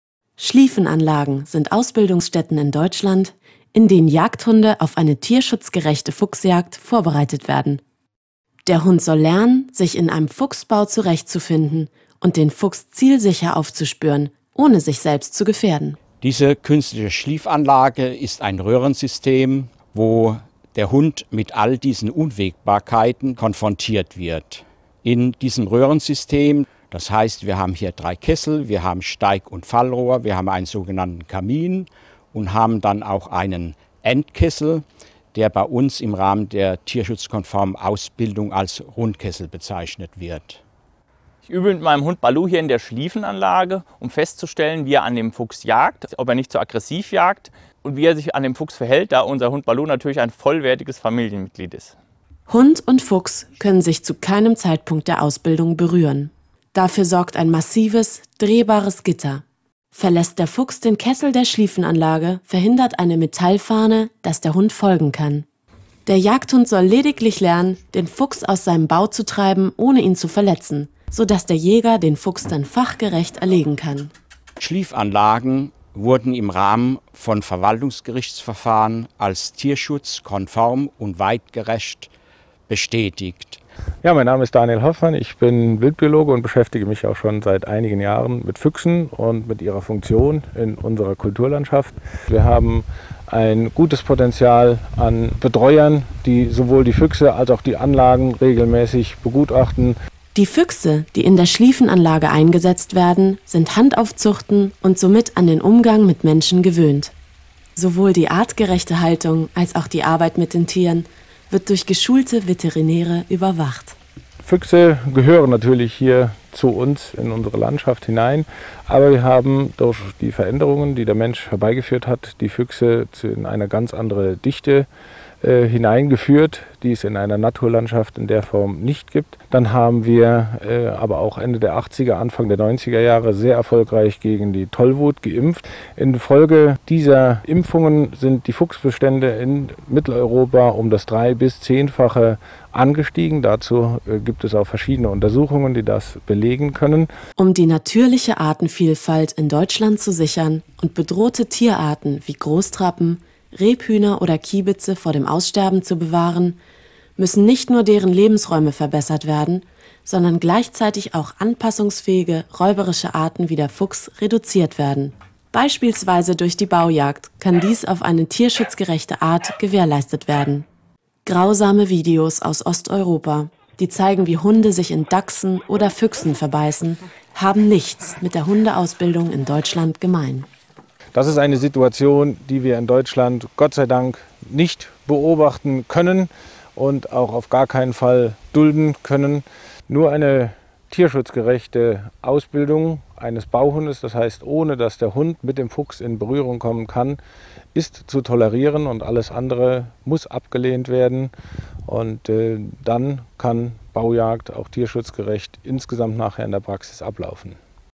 Schliefenanlagen sind tierschutzgerecht
Für die Baujagd auf Füchse oder Dachse muss zunächst die jagdliche Brauchbarkeit von Hunden getestet werden. Und das ist wichtig: Der Hund lernt dabei, dass er Abstand vom Fuchs halten muss, um sich und das Tier nicht zu gefährden. Durch die Arbeit am lebenden Tier lassen sich aggressive oder ängstliche Hunde frühzeitig erkennen. In der sogenannten Schliefenanlage findet die Ausbildung mit handzahmen Füchsen statt. Dort werden die Verhältnisse eines Fuchsbaus simuliert.